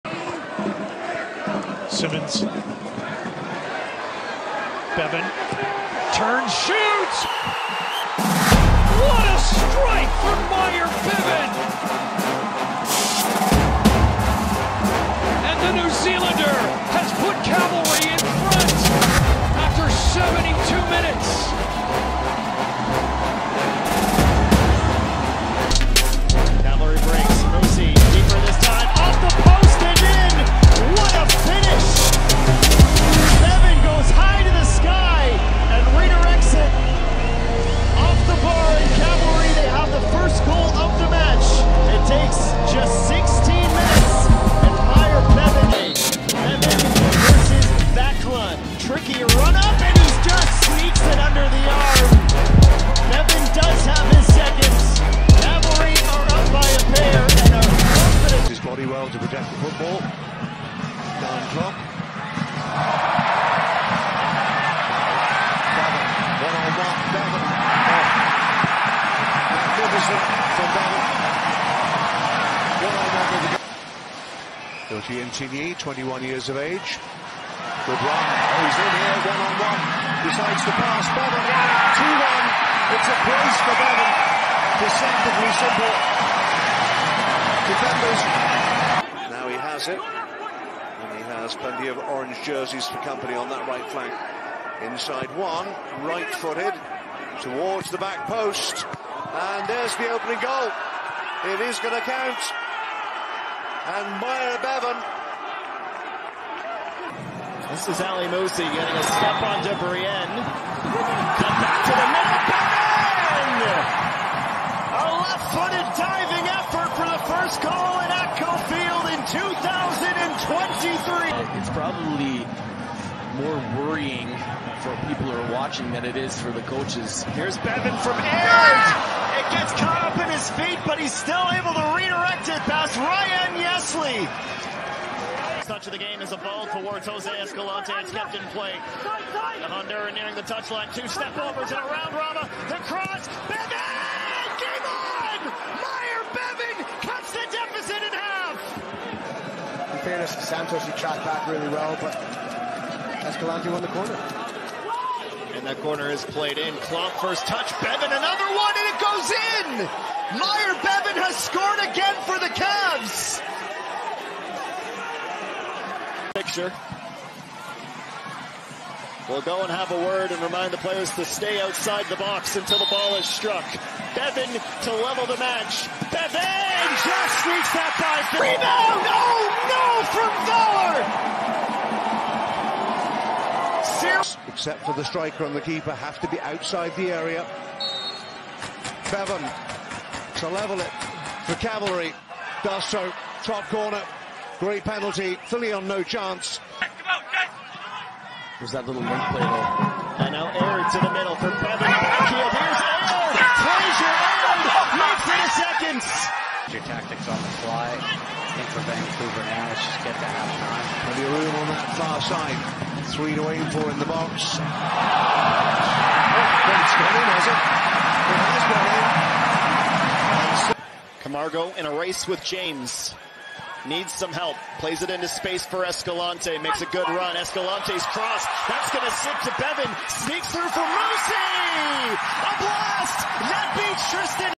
Myer Bevan - All time goals and assists for Cavalry FC
14 Goals
3 Assists
1890 Minutes
28 Appearances in all comps